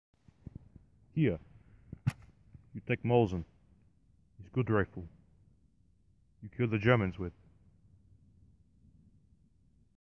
Comrade Cat Part 1
Mr.Bigglesworth has enlisted in the glorious Russian army.